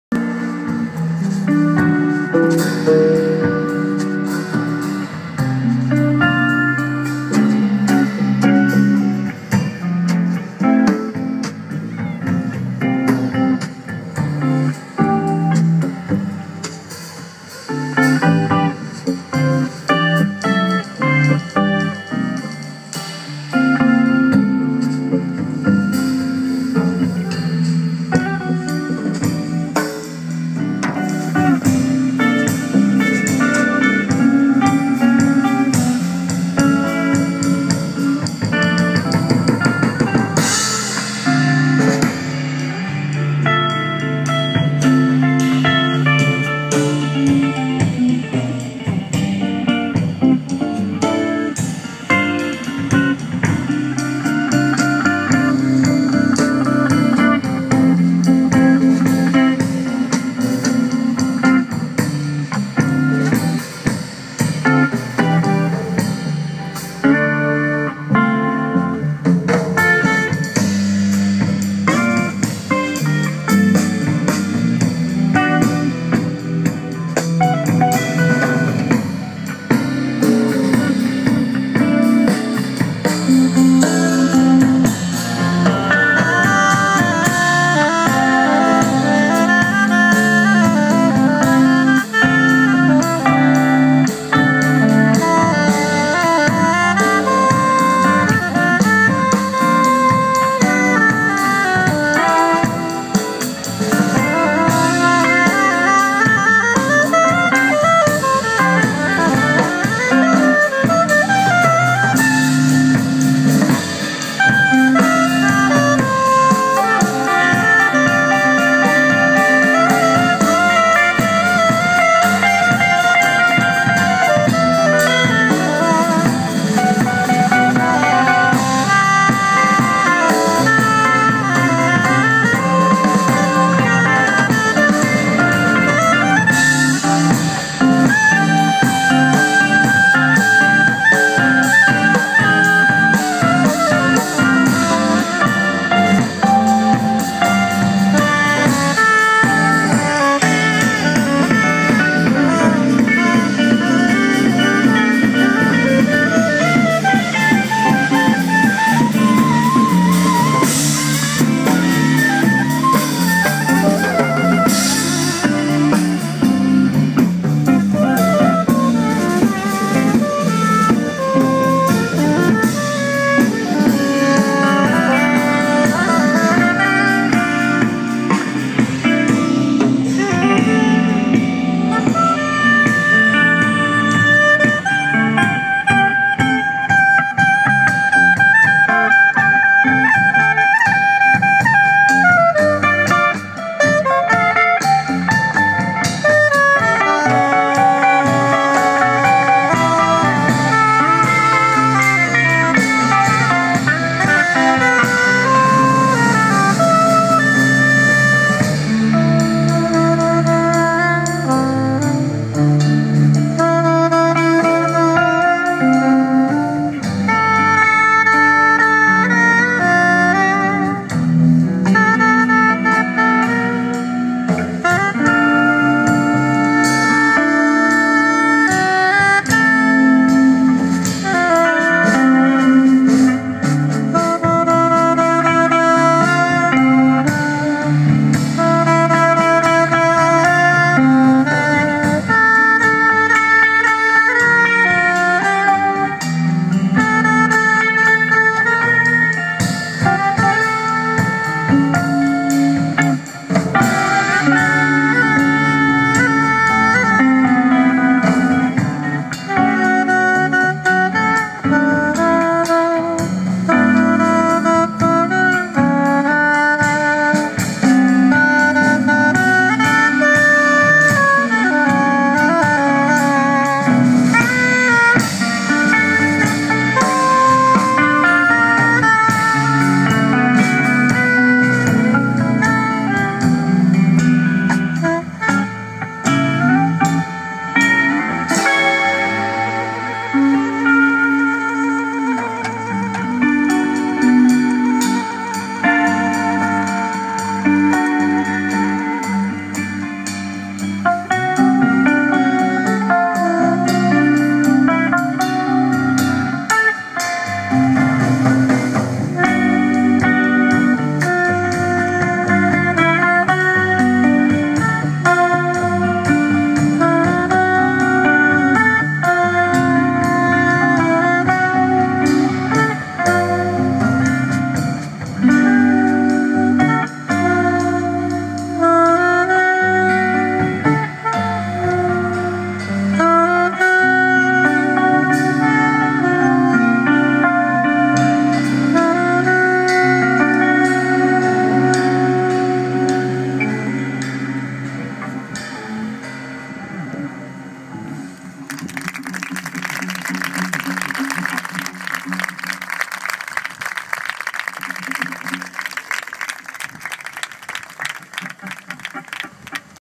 Jazz in spiaggia
7 Agosto 2013 - Badesi (Sardegna), spiaggia Li Junchi. Al tramonto assistiamo ad un bellissimo concerto del "Decoder Quartet" con Enzo Favata al sassofono, Marcello Peghin alle chitarre, Danilo Gallo al basso e U.T. Gandhi alle percussioni. Quello che ho ripreso è il primo pezzo, eseguito proprio durante il tramonto. Grande emozione! In lontananza l'isola dell'Asinara.